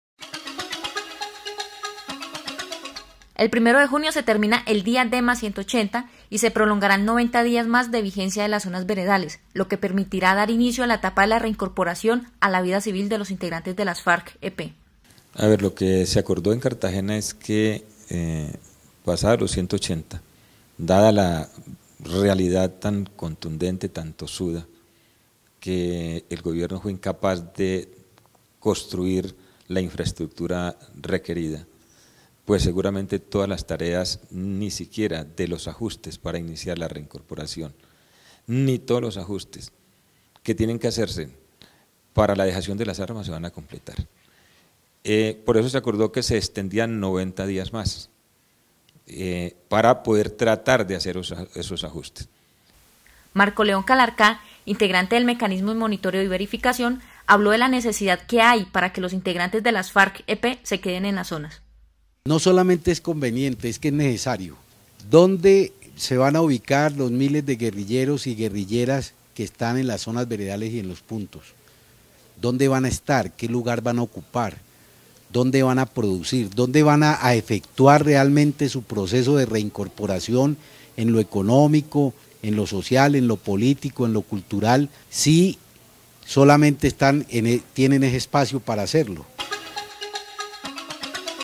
Zonas Veredales se extenderán 90 días más revelaron las FARC-EP
En entrevista a NC Noticias los comandantes de las FARC-EP Pastor Alape y Marco León Calarcá revelaron que las Zonas Veredales se extenderán 90 días más debido a los incumplimientos en la implementación de los acuerdos.